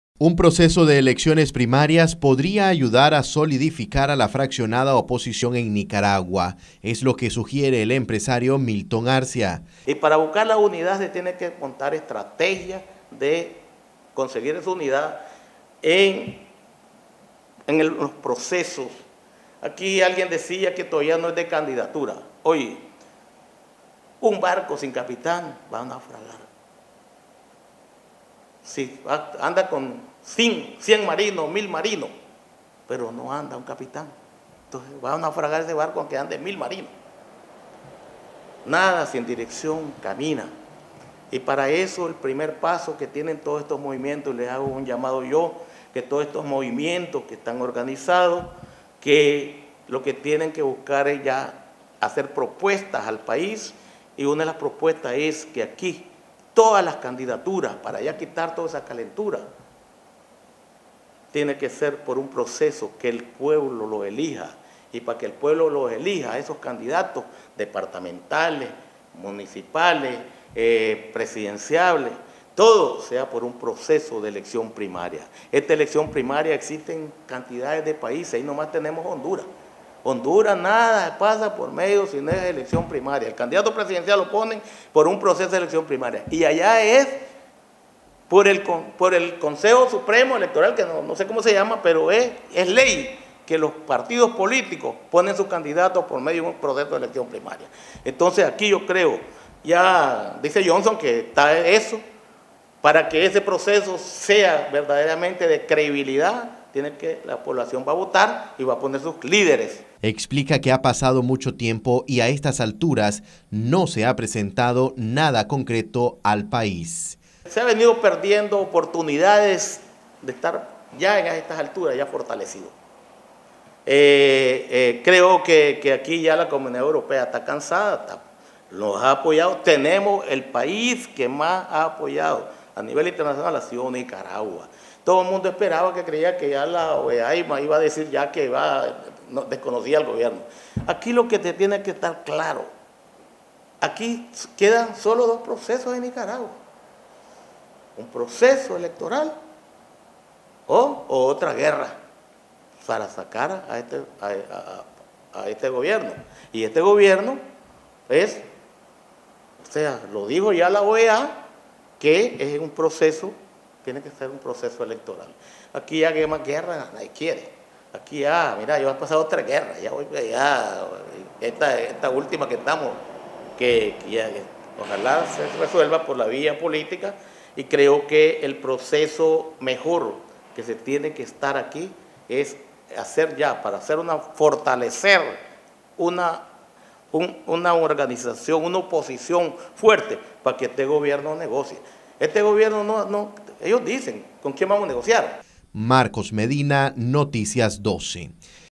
Empresario recomienda elecciones primarias para consolidar la oposición en Nicaragua
EMPRESARIO MILTON ARCIA RECOMIENDA ELECCIONES PRIMARIAS PARA AYUDAR A LA CONSOLIDACIÓN DE LA OPOSICIÓN EN NICARAGUA